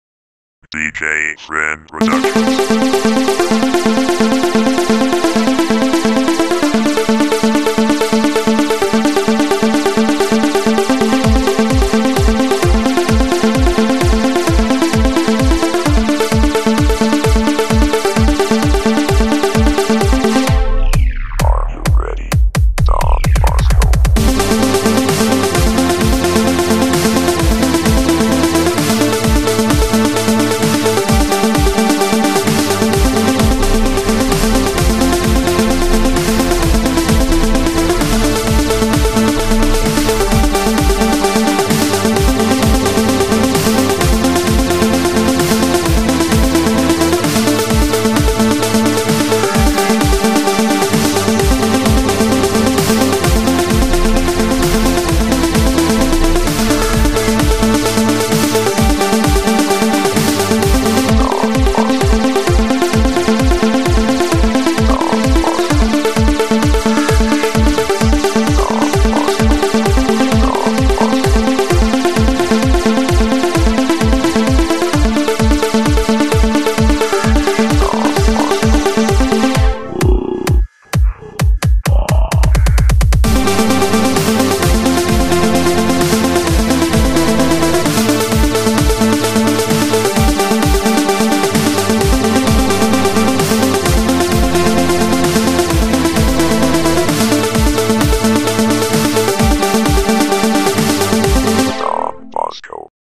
Don Bosco dubstep
i made this under 5 minutes
it was for a school project